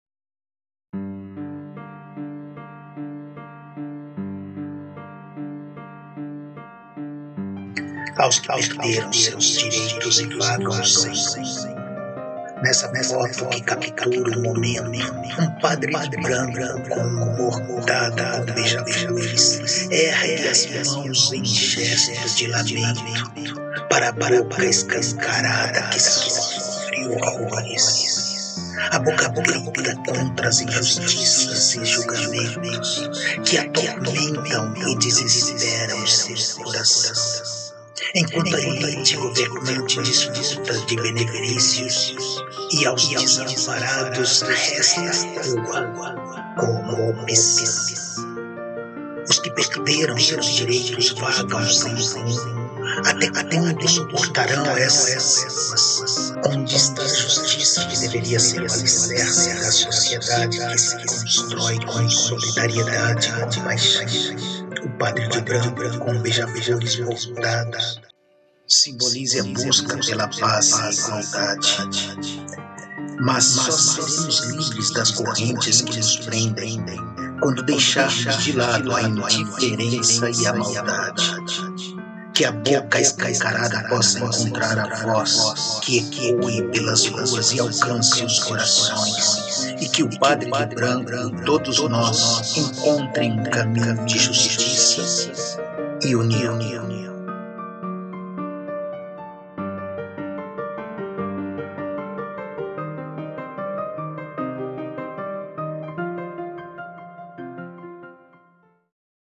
AOS QUE PERDERAM SEUS DIREITOS E VAGAM SEM RUMO _ EDUARDO BRODOWISKI _
AOS QUE PERDERAM SEUS DIREITOS E VAGAM SEM RUMO

NESSA FOTO QUE CAPTURA UM MOMENTO, UM PADRE DE BRANCO COM UM BORDADO COM BEIJA-FLORES, ERGUE AS MÃOS EM GESTO DE LAMENTO, PARA A BOCA ESCANCARADA QUE SOFRE HORRORES.

A BOCA GRITA CONTRA AS INJUSTIÇAS E JULGAMENTOS, QUE ATORMENTAM E DESESPERAM SEU CORAÇÃO, ENQUANTO A ELITE GOVERNANTE DESFRUTA DE BENEFÍCIOS, E AOS DESAMPARADOS RESTA A RUA COMO OPÇÃO.

OS QUE PERDERAM SEUS DIREITOS VAGAM SEM RUMO, ATÉ QUANDO SUPORTARÃO ESSA SITUAÇÃO? ONDE ESTÁ A JUSTIÇA QUE DEVERIA SER O ALICERCE, DA SOCIEDADE QUE SE CONSTRÓI COM SOLIDARIEDADE E COMPAIXÃO?

O PADRE DE BRANCO COM BEIJA-FLORES BORDADOS, SIMBOLIZA A BUSCA PELA PAZ E IGUALDADE, MAS SÓ SEREMOS LIVRES DAS CORRENTES QUE NOS PRENDEM, QUANDO DEIXARMOS DE LADO A INDIFERENÇA E A MALDADE.

QUE A BOCA ESCANCARADA POSSA ENCONTRAR A VOZ, QUE ECOE PELAS RUAS E ALCANCE OS CORAÇÕES, E QUE O PADRE DE BRANCO E TODOS NÓS, ENCONTREM UM CAMINHO DE JUSTIÇA E UNIÃO.

                                 _ EDUARDO BRODOWISKI  _         07/05/2023